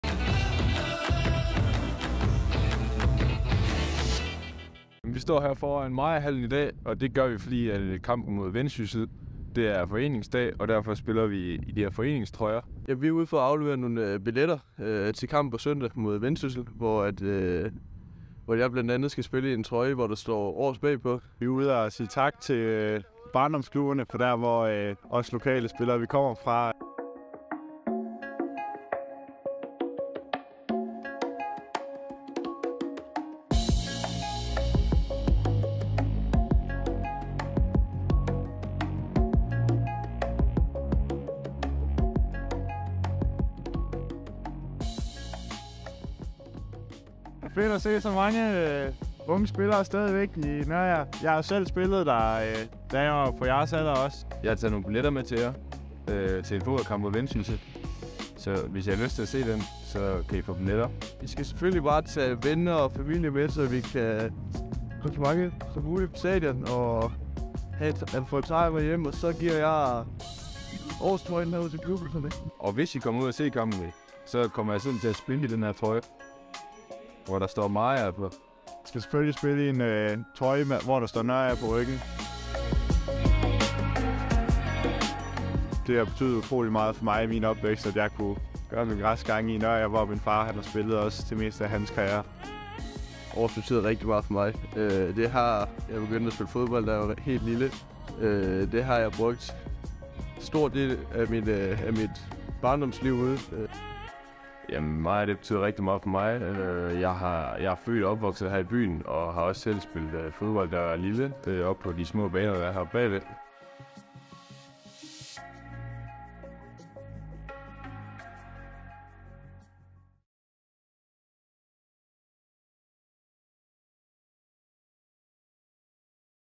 Foreningsdag til Hobro IK - Vendsyssel FF
På søndag afholder vi foreningsdag og i den anledning har Laurs Skjellerup, Mads Freundlich og Frederik Dietz været ude i deres barndomsklubber, for at afleverer billetter til de børn der har deres græsgange i selv samme klubber.

Vi håber at se en masse på DS Arena på søndag, hvor vi skal hylde lokalsamfundet og breddeklubberne. 💛